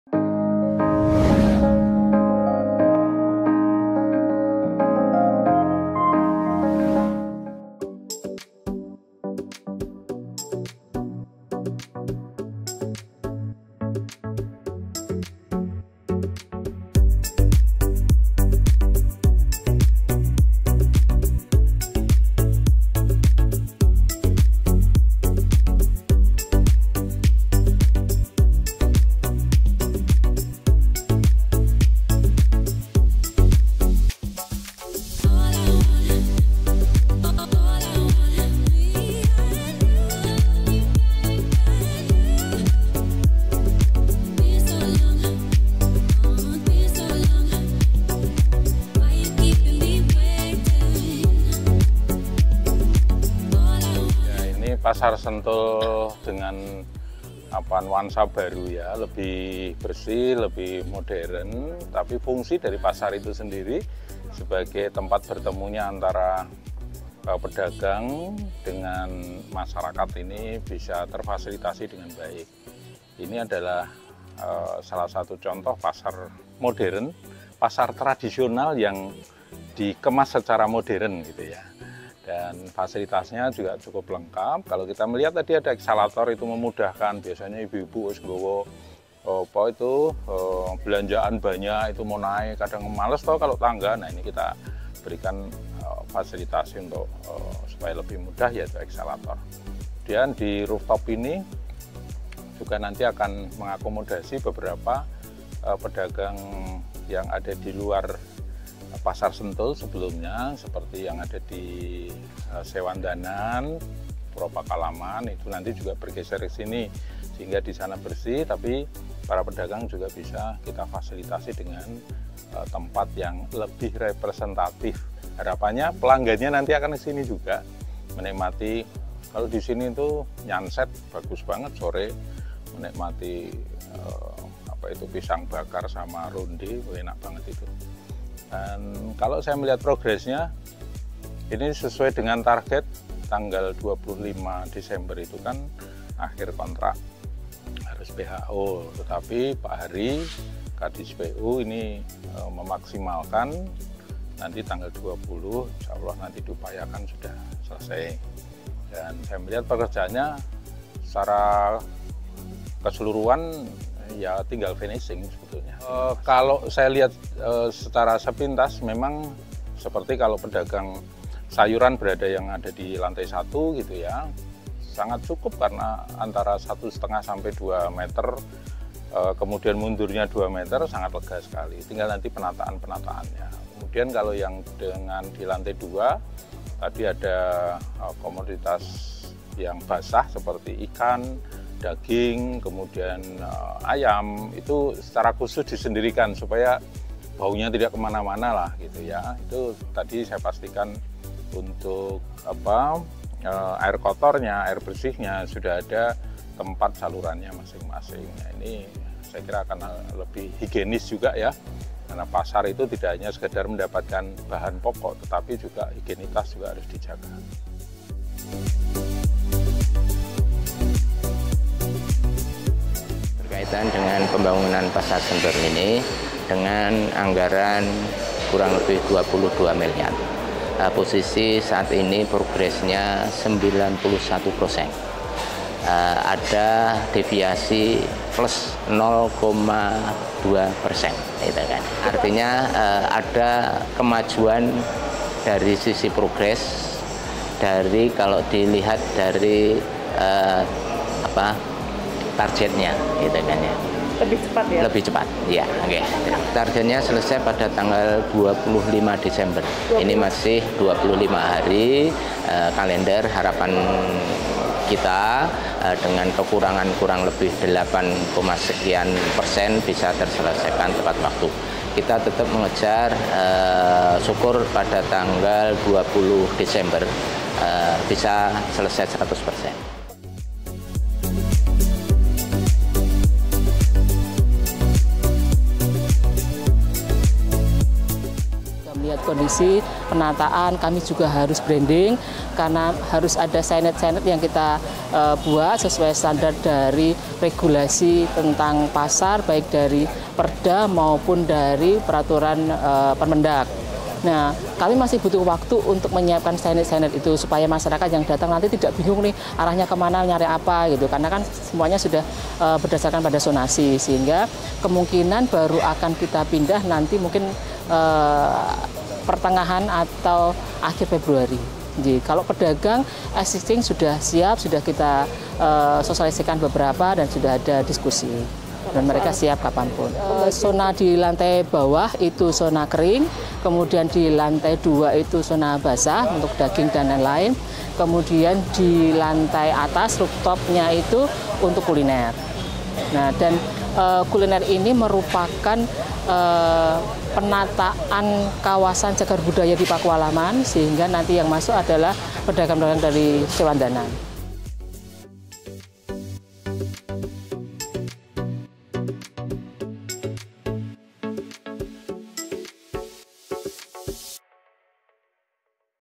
Pembangunan Pasar Sentul Yogya Rampung 91% - Terowongan Waktu Menuju Modernitas! - Warta YK
🌟 Kali ini di Warta YK, kita bawa kabar seru dari Pemkot Yogya! Pembangunan Pasar Sentul memasuki fase final, dan kita pastikan semuanya rampung tepat waktu! 🏗️✨ Dalam kunjungan Penjabat Wali Kota Yogyakarta, Singgih Raharjo, kita saksikan progres 91% menuju pasar modern nan bersih.

Tanggal target 25 Desember semakin mendekat, namun optimisme ada di udara! Tanggal 20 Desember diupayakan selesai 100%. Singgih menjamin semua fasilitas untuk pedagang dan pengunjung akan tersedia. Rooftop, toilet, eskalator, semuanya dirancang agar Pasar Sentul nyaman dan higienis. 🛒🚿

Pasar tradisional yang terangkum dalam konsep modern dengan sentuhan arsitektur Indis ini, bakal menjadi magnet baru di Jogja. Railing, eskalator, dan koneksi listrik sedang dipersiapkan, sambil mengejar target selesai tepat waktu. 📆🏙️

Jadi, tunggu apa lagi? Saksikan perubahan menakjubkan ini dan ikuti terus update Warta YK untuk info terbaru seputar Pasar Sentul! 🎥👍

🔗 Temukan informasi menarik lainnya di playlist Warta YK kami: [Tambahkan Link Playlist di Sini]

🌈 Terima kasih sudah menjadi bagian dari YKTV! Jangan lewatkan video seru berikutnya! 🌟